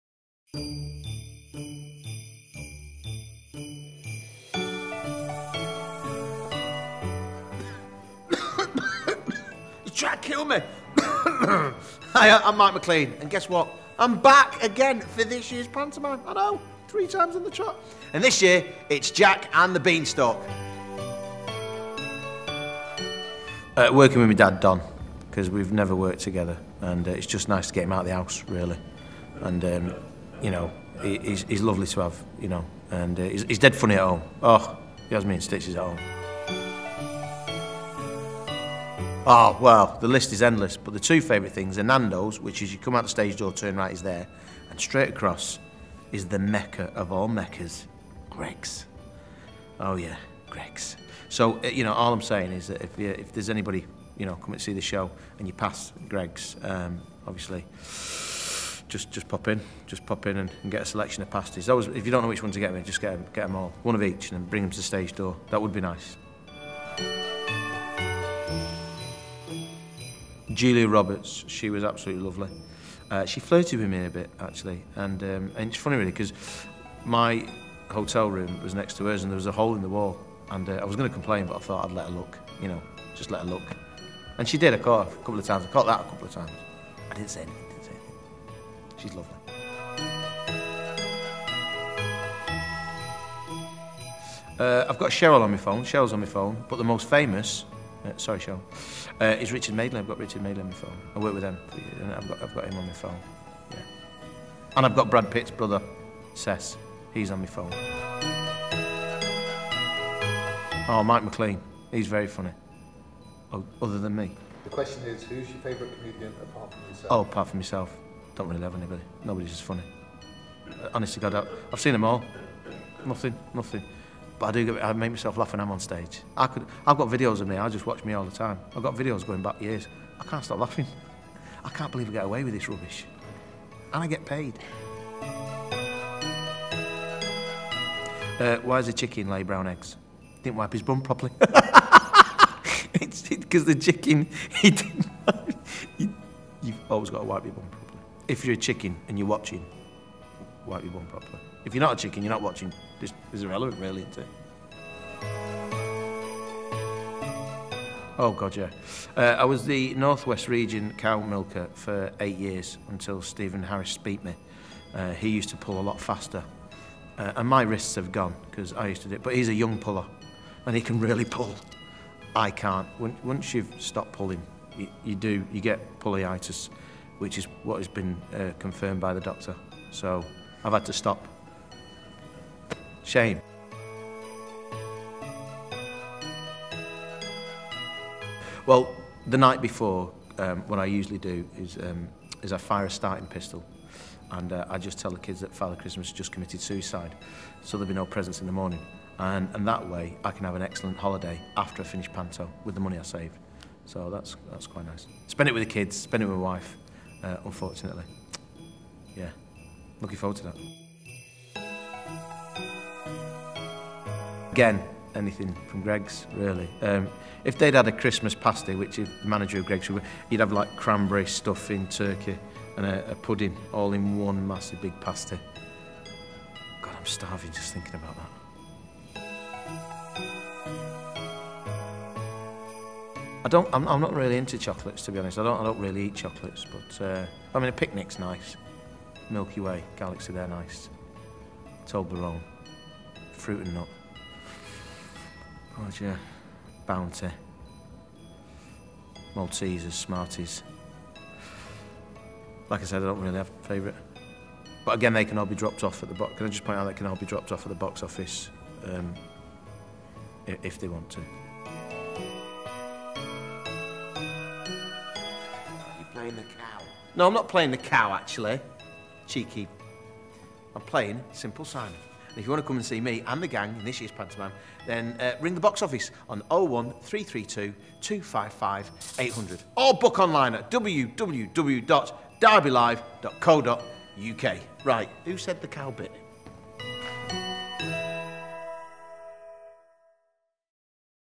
Q & A with Derby LIVE Panto Favourite Mike McClean
Here's a little question and answer session with Derby LIVE Panto Favourite Mike McClean starring in our pantomime, Jack and the Beanstalk, starring Cheryl Fergison, Marcus Collins and Don Maclean. See it at the Assembly Rooms, Derby from Wed 4 Dec - Sun 5 Jan.